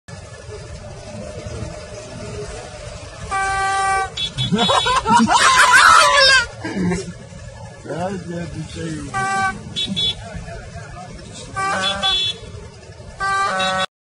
Grizzly Bear Blowing Trumpet.
Saw this on Twitter and this is something you gotta see to believe!  The bears are becoming more intelligent.